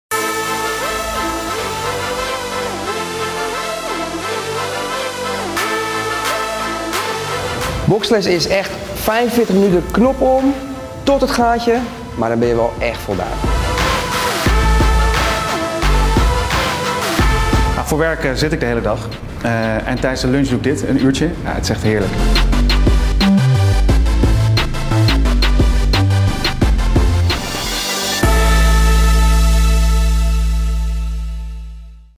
Group classes at Clubsportive - Brooklyn Fitboxing
An addictive mix of boxing, kickboxing, and Muay Thai. Alternate between a technical choreography of kicks and punches, squats, jumping jacks and push ups. The instructor and the music will get you through the class, and the big screen shows a demo of all the exercises.

Are you ready for this full body workout?